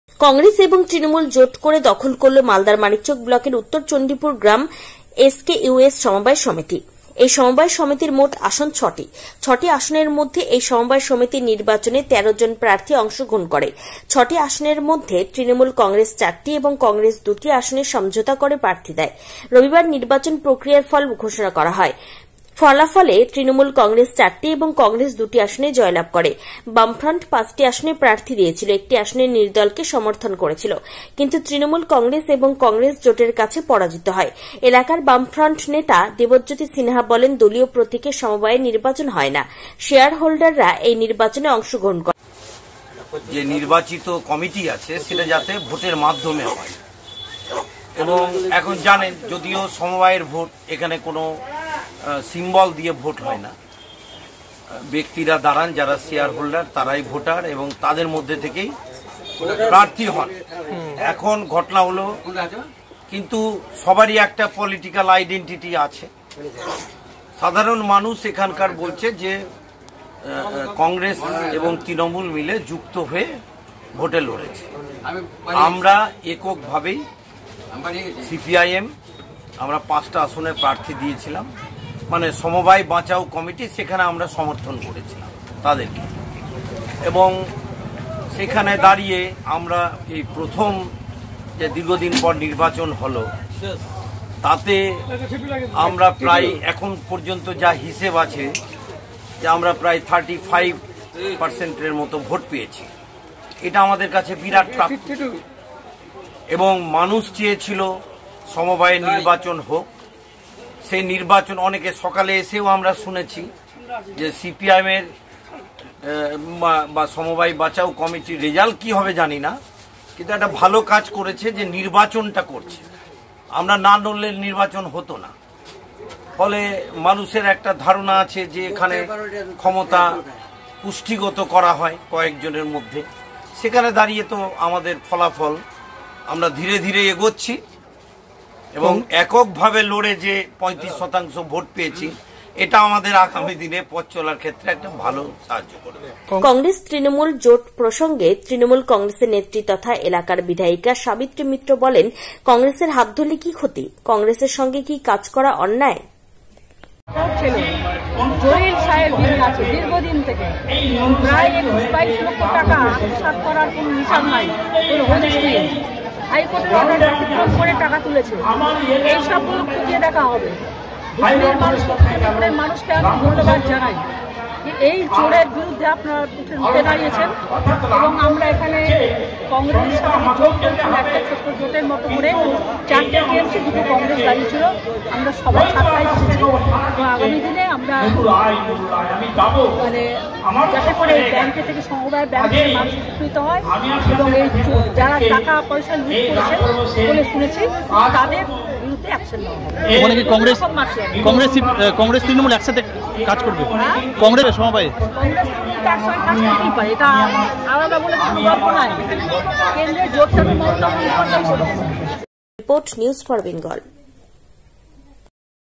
Cong+TMC Alliance won Samabay Samity Election:SKUS সমবায় সমিতি নির্বাচনে জয় কংগ্রেস, তৃণমূল জোটের।
কংগ্রেস তৃণমূল কংগ্রেসের জোট করে দখল করল মালদার মানিকচক ব্লকের উত্তর চন্ডিপুর গ্রাম SKUS সমবায় সমিতি।